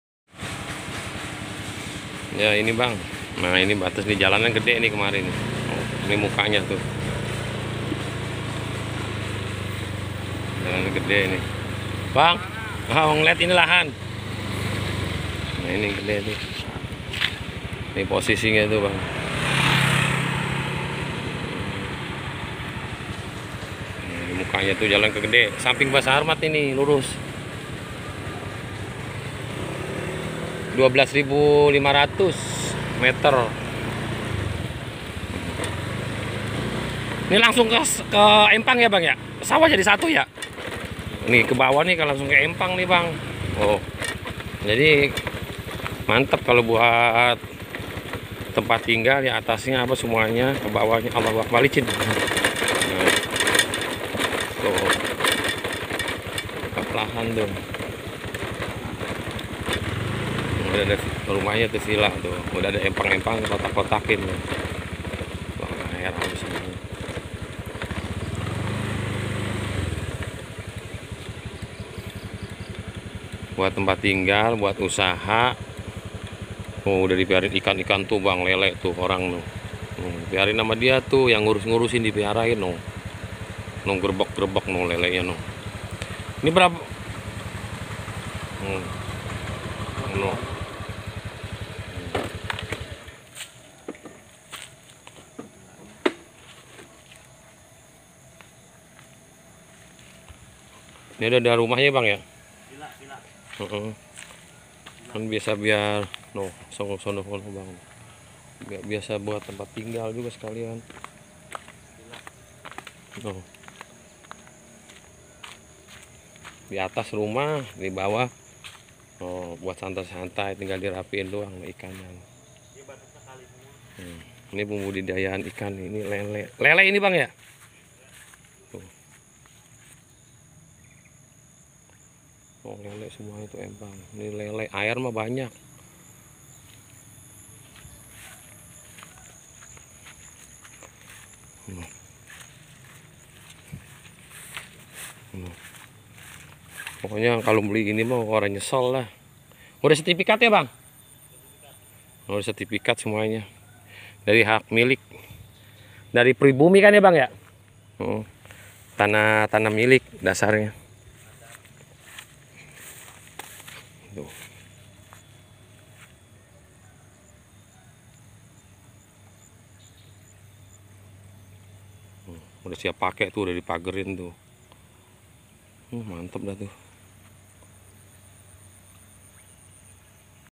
Jual Tanah paling murah 12500
Babakan Ciseeng Bogor Jawa barat..dekat tol Serpong Tanggerang .tol Bojong Sari .tol sawangan .tol Bogor ..masuk Bogor barat...0895323008778//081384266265